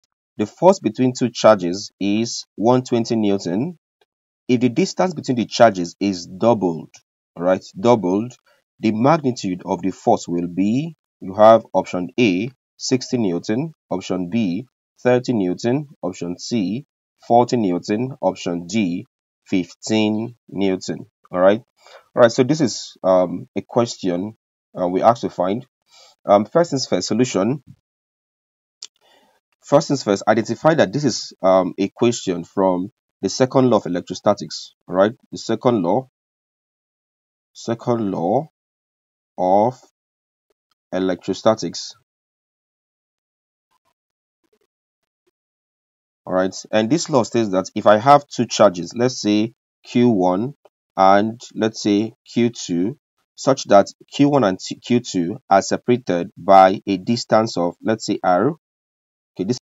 Force Between Charges: Electrostatics JAMB Physics Example. How to Find The Force Between Charges.
In this Physics tutorial video, I discuss and explain Force Between Charges: Electrostatics JAMB Physics Example. How to Find The Force Between Charges. You will learn how to apply the second law of electrostatics in solving problems in Physics.